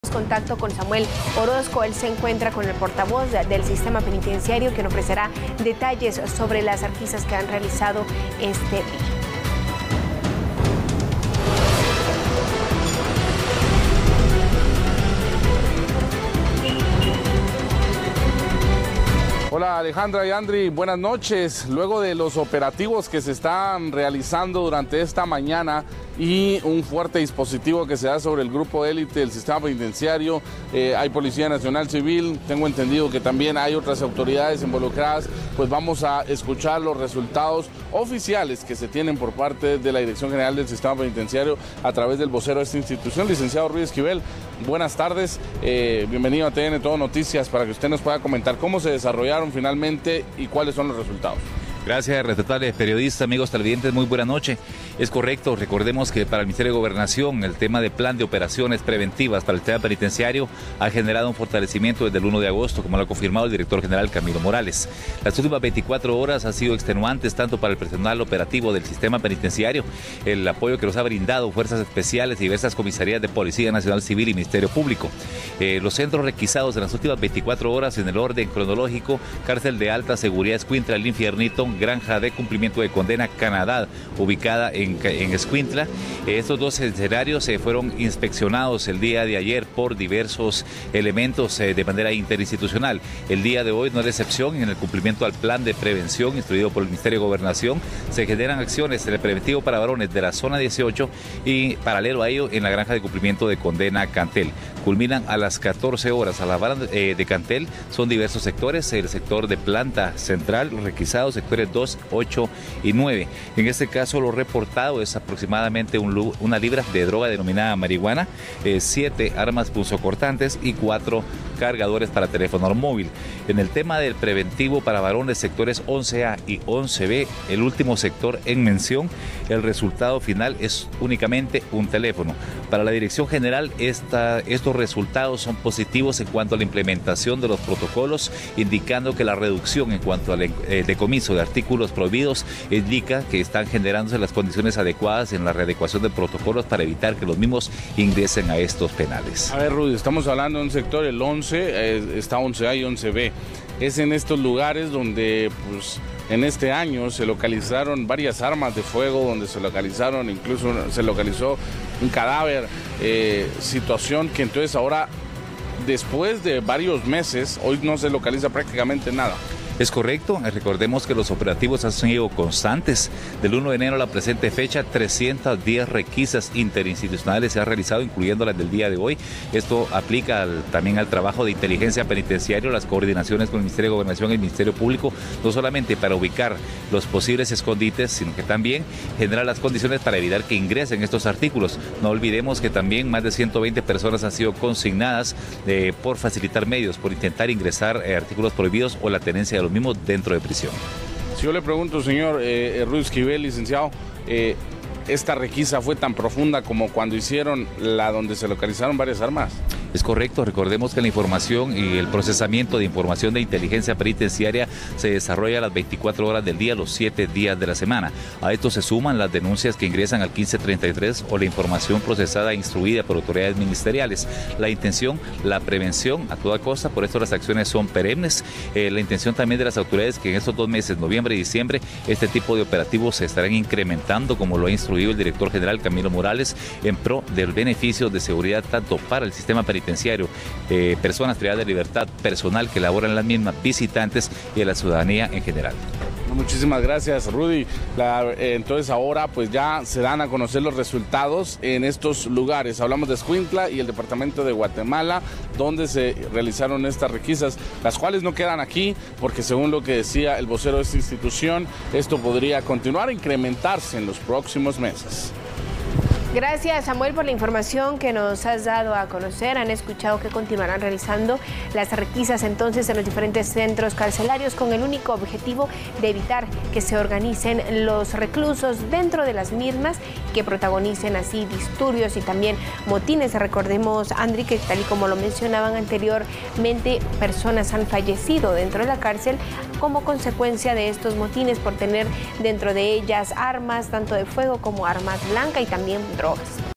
Ilícitos dentro de cárceles nacionales
Sistema Penitenciario da detalles de las requisas en la capital y Quetzaltenango

El portavoz del Sistema Penitenciario dio detalles sobre las requisas que han realizado en la capital y Quetzaltenango durante la mañana.